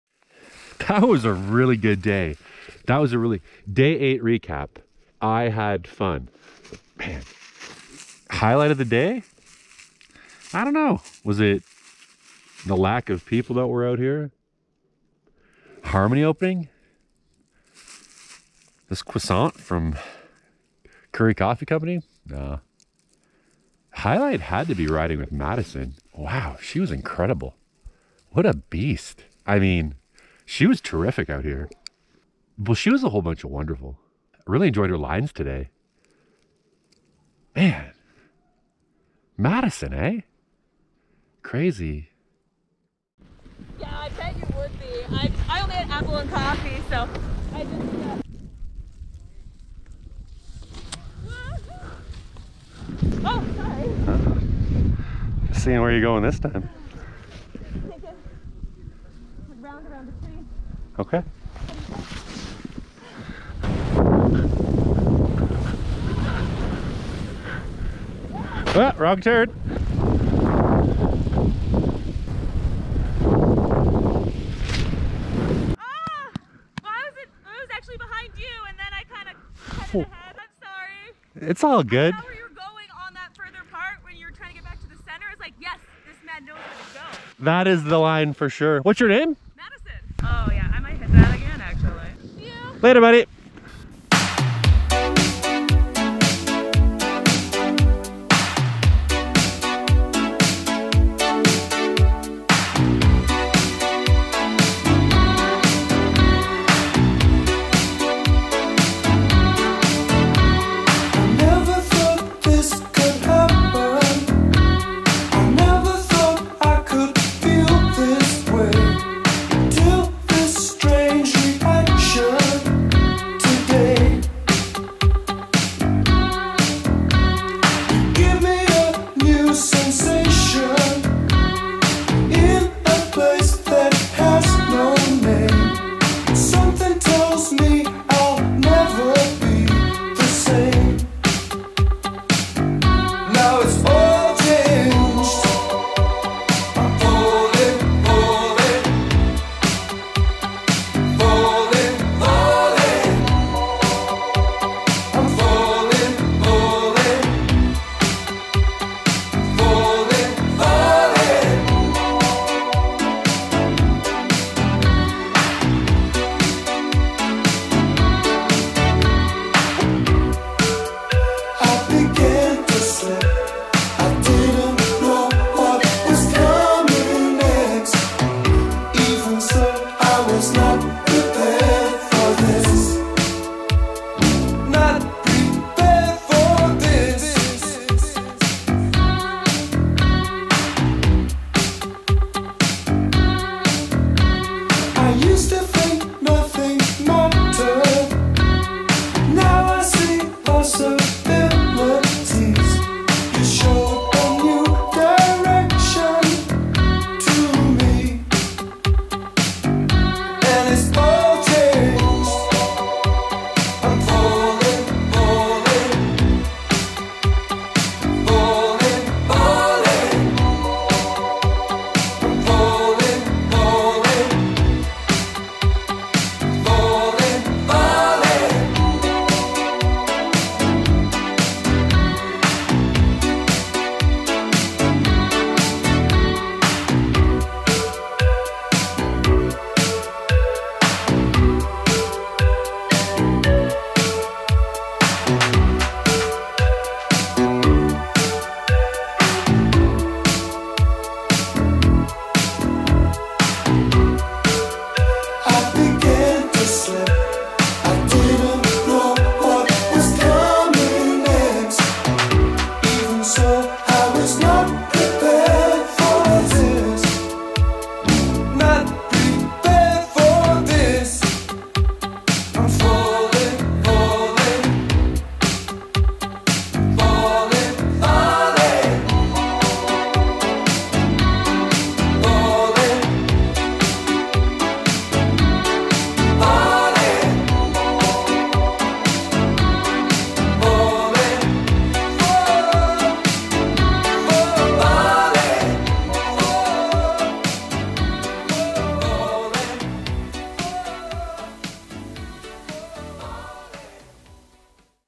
Whistler Early Winter Riding 2020 - Day 8 - Chasing Madison
Combine the opening of Harmony Zone with a powder day...unreal results!

Lap after lap of fun down the line of the Harmony Chair!

I had the pleasure of riding with Madison from Whistler for some laps...more like chasing Madison as she's a burner🔥

Lots of style, smiles and laughs...it was a dream to ride with such a terrific person!

Coffee and Apples!

As always, I hope you enjoyed the video and thanks for dropping by the channel!